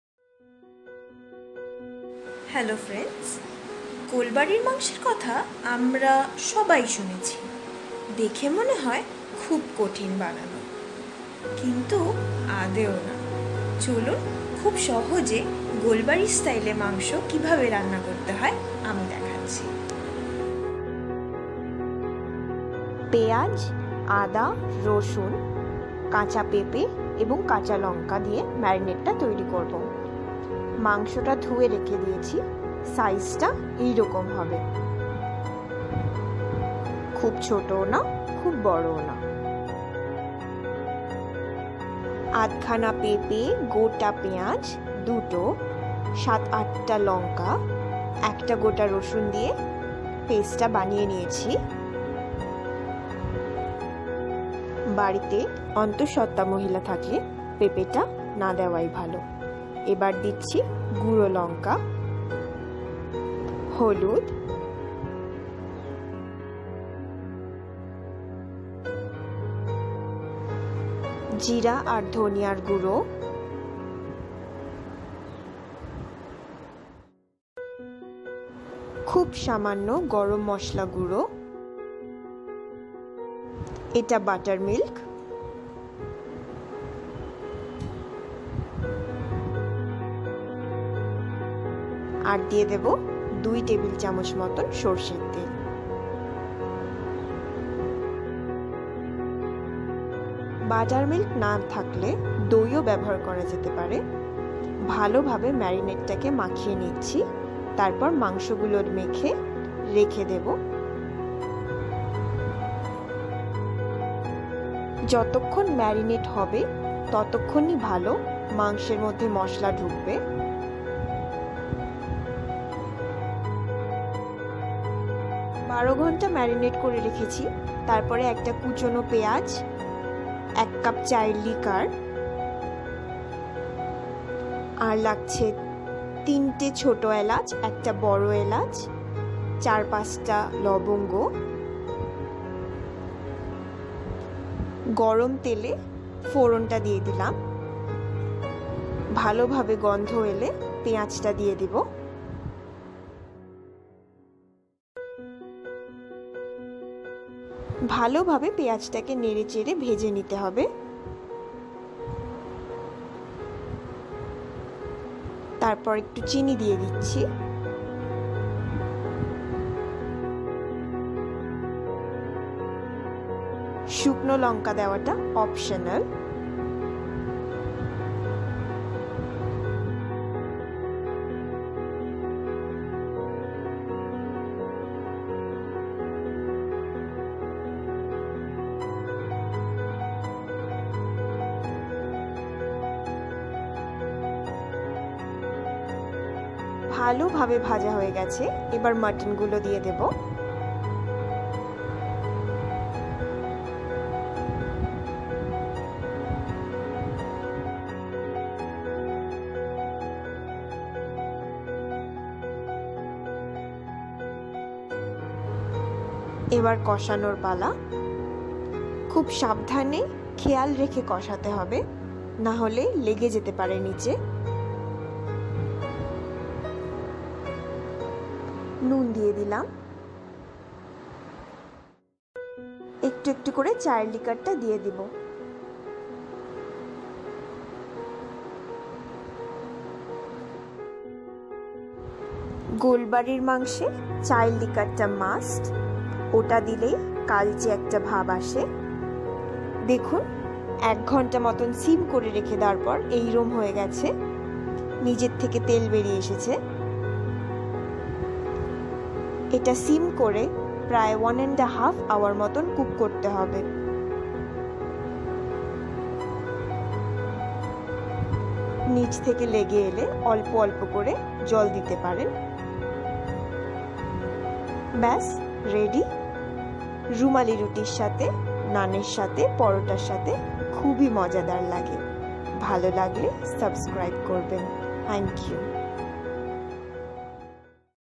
Golbarir Kosa Mangso Recipe
Easy way to cook this authentic Golbari Mangso